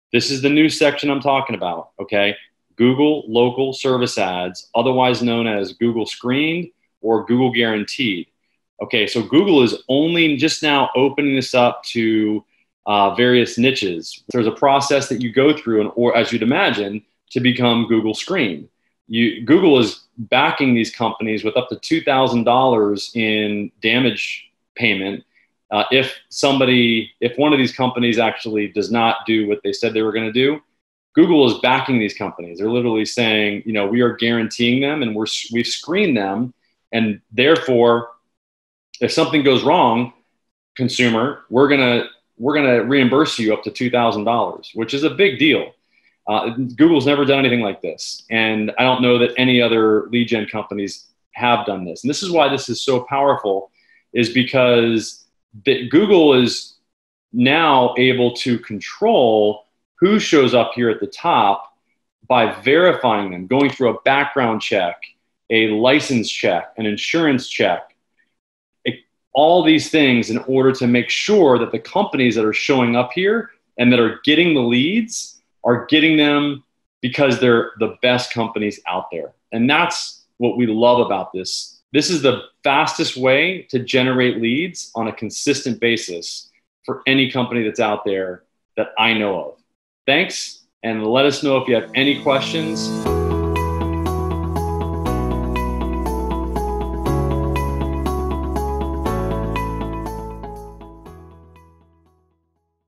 Google Guaranteed Benefits: $2,000 of Google Support
The Google Guaranteed program was designed to increase customer confidence in from Google Ads. Businesses that have been certified by Google Guaranteed Services will have a green and white check mark displayed next to their ads in search results.